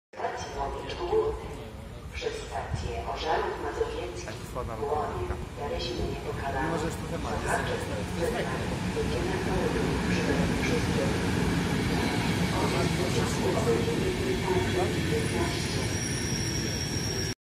EU44-009 z pociągiem IC wjazd na stacji warszawa Zachodnia
Wjazd pociągu IC prowadzonego przez lokomotywę eu-44 najprawdopodobniej jest to Berlin-Warszawa-express
Skład skierowany jest w stronę Warszawy Wschodniej.